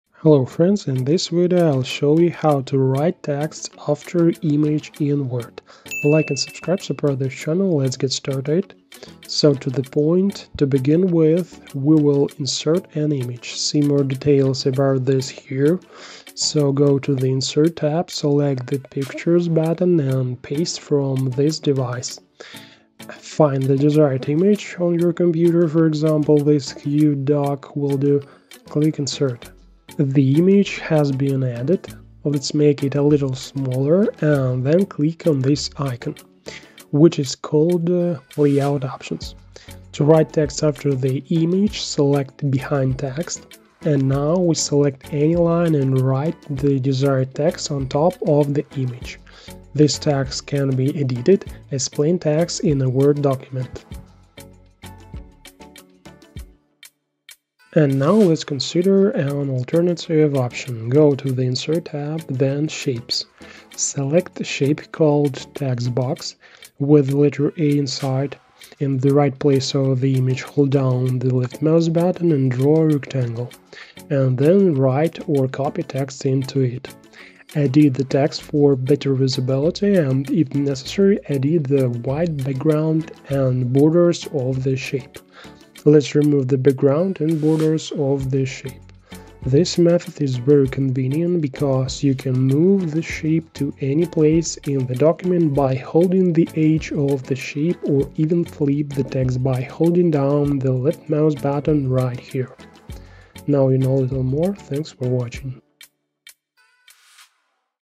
How To Write Text After Image In Word
00:00 - Hello
00:10 - The first way
00:56 - The alternative way